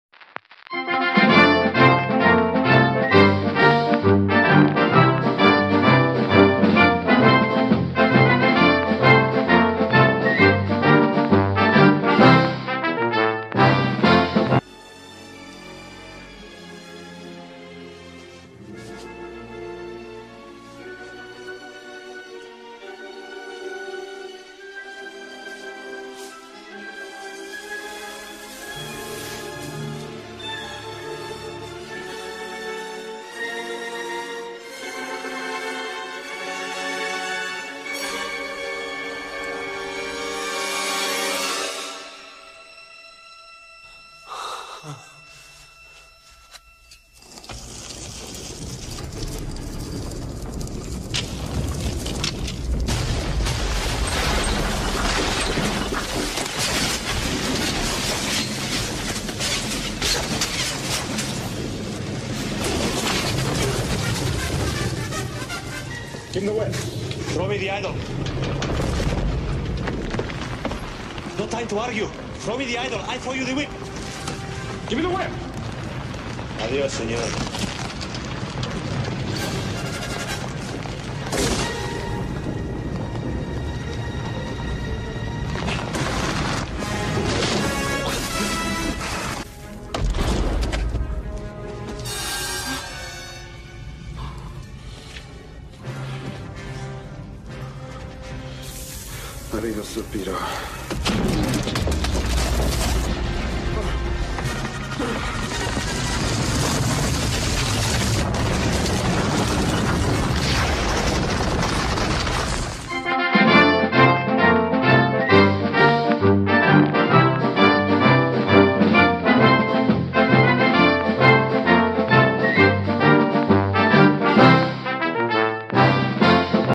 Anteater Theater: Indiana Jones
Missing Anteater theater? No fear! It has returned in the form of Indiana Jones and the Raiders of the Lost Ark! Relive the infamous opening sequence, recreated by The Hill! enjoy!